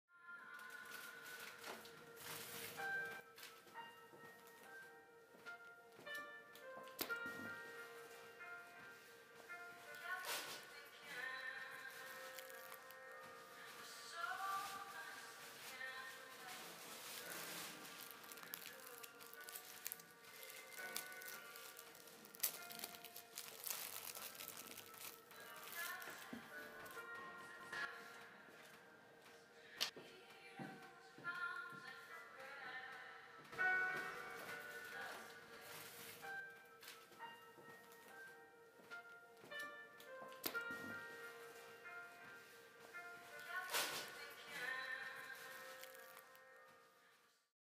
extra buttons
Two replacement buttons are put of their silk and plastic bags and are exposed on the table.
This video-documentation is redundant; it is more than we need, for we don´t need it. It is extra.
Just like the buttons are.